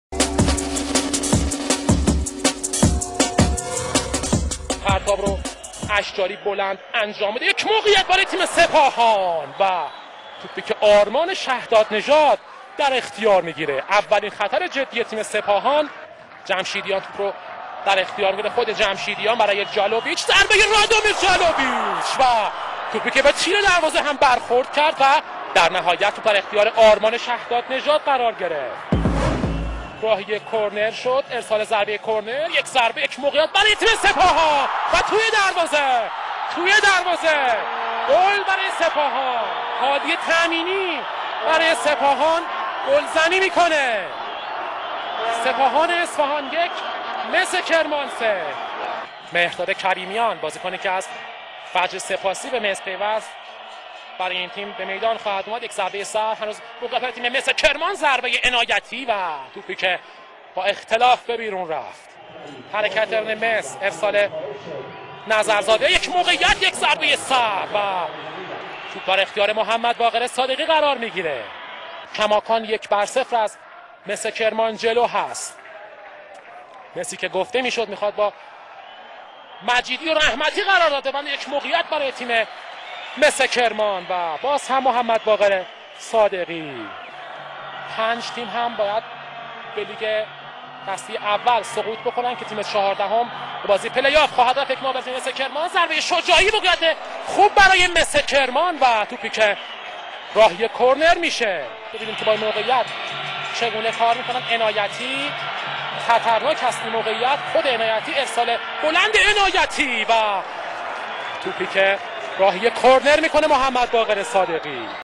[IPL 12/13] Sepahan - Mes Kerman (Match Highlights)
IPL 12/13 | Sepahan 1 - 0 Mes Kerman
47' [1-0] Hadi Tamini